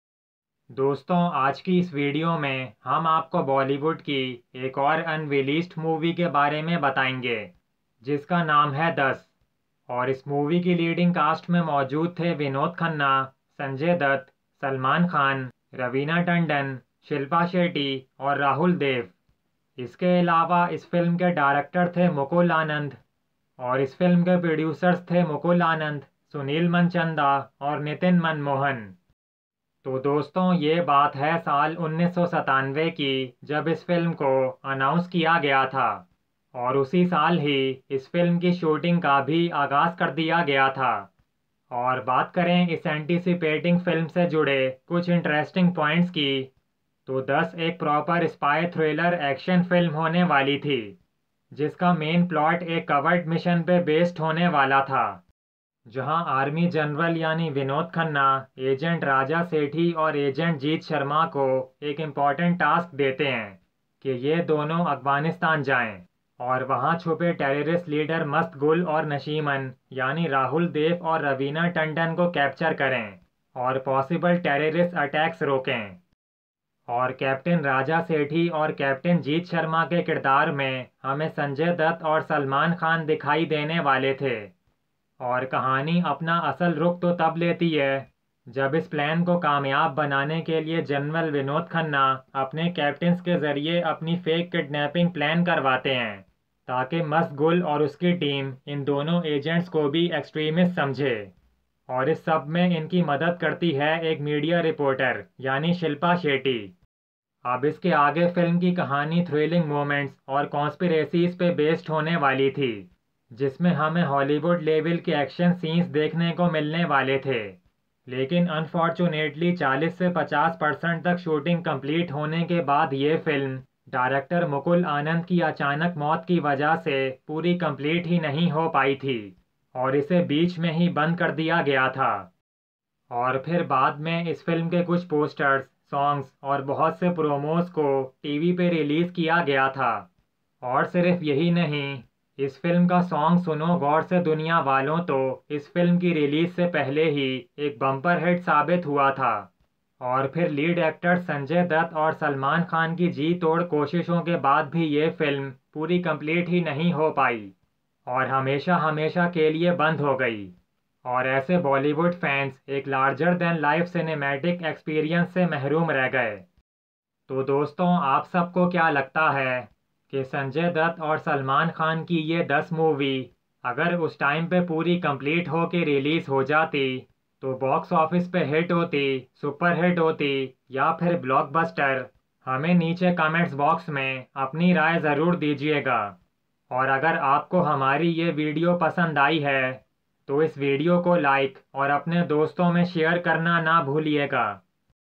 DUS - Vinod Khanna , Sanjay Dutt And Salman Khan Unreleased Bollywood Movie Full Details | Rahul Dev
DUS 1997 Vinod Khanna , Sanjay Dutt And Salman Khan Unreleased Bollywood Movie Full Details | Rahul Dev | Raveena Tandon | Shilpa Shetty

Director: Mukul Anand
Producers: Mukul Anand , Sunil Manchanda & Nitin Manmohan